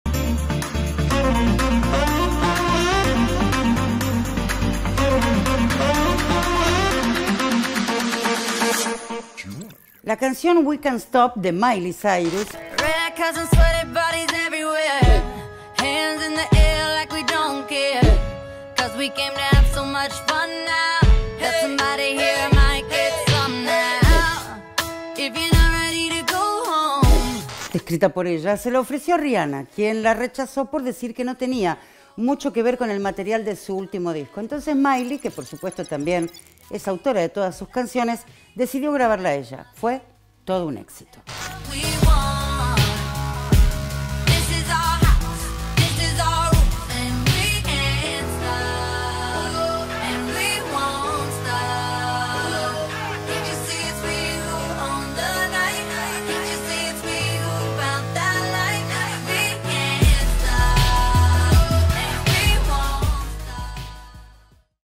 SOUNDCRACK: CURIOSIDADES DEL MUNDO DE LA MÚSICA
¿SABÍAS QUE... RIHANNA RECHAZÓ UNA CANCIÓN QUE MILEY CYRUS HIZO UN EXITO?